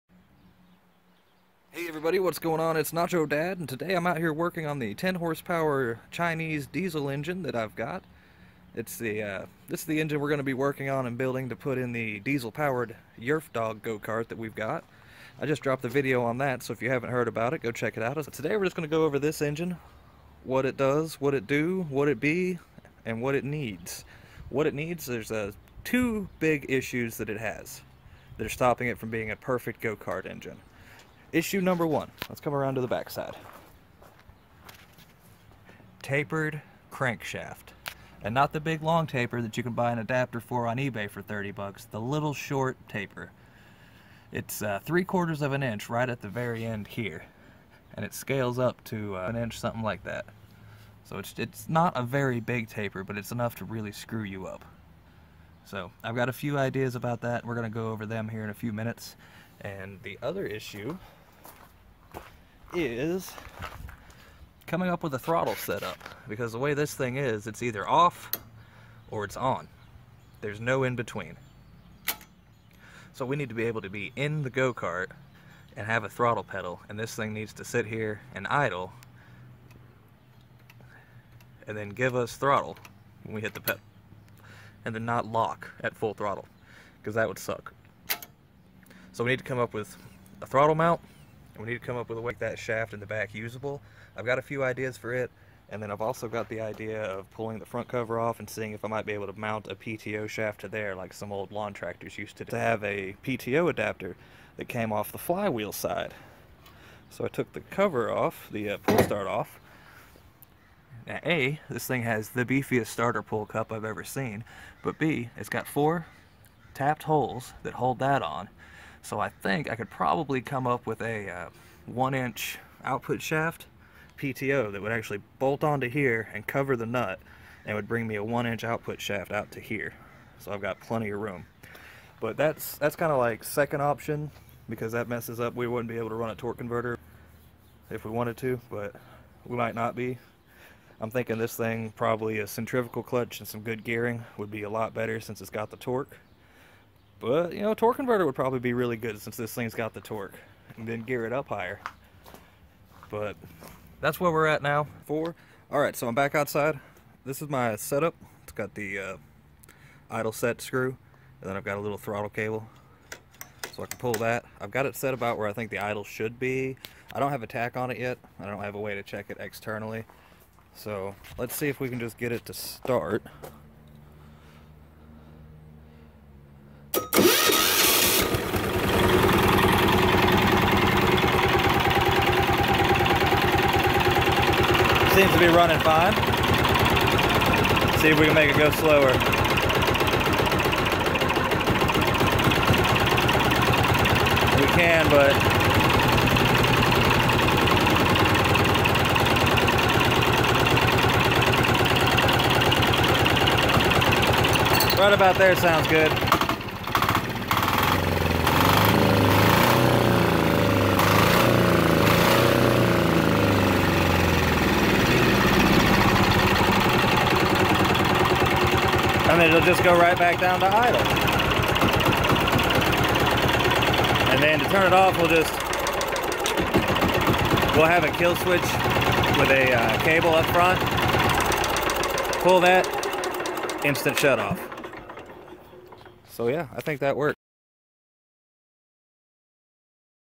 10 HP Diesel DIY Idle and Throttle set up |Diesel Go Kart Build
First I want to say I'm sorry for the horrible editing and quality of this video.  I have ordered a new battery for my good camera and will be using it for most of my videos from now on.

Now that we have the idle and throttle set up figured out and working I will be working on converting the taper shaft to a 1 inch keyed shaft.